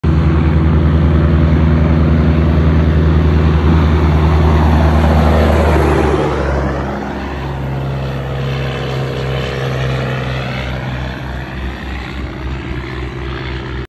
Kenworth W900 Loaded Vs Kenworth W900 Empty Up 8% Grade in the Badlands
The quickest, cheapest and easiest way to add horsepower to your truck is to unload the trailer.  I’ve passed and been passed on this hill others many times.  I drove a 379 Peterbilt with a Signature 600 in it and it could pass empty 400hp trucks going uphill when it was loaded.  All claims are subject to being slightly BS or inaccurate due to poor memory.